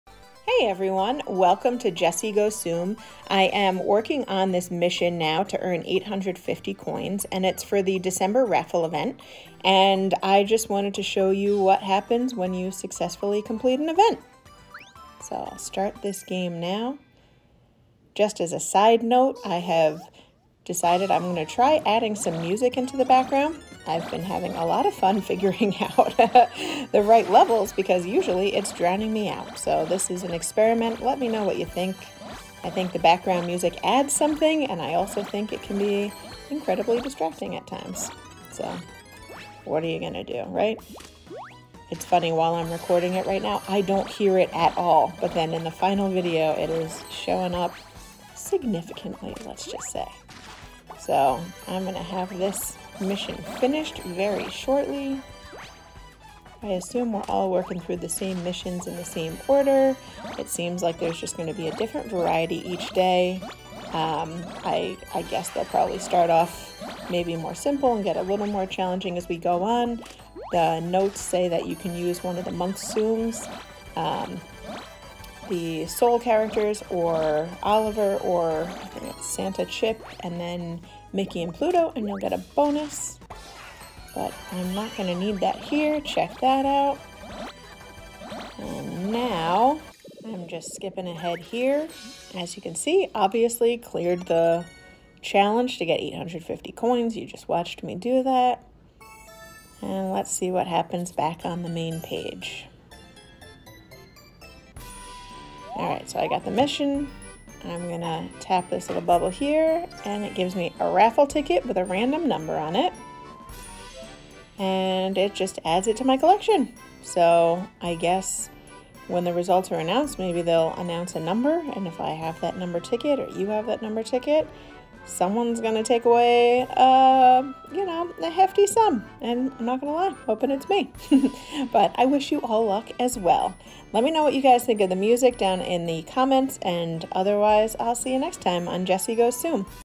December Raffle Event
Here's a quick overview of what's going on in the December Raffle Event. Remember, there are new cards each day, each with multiple missions you can complete for a raffle ticket, and the cards can *only* be played on that day!

This video is for all audiences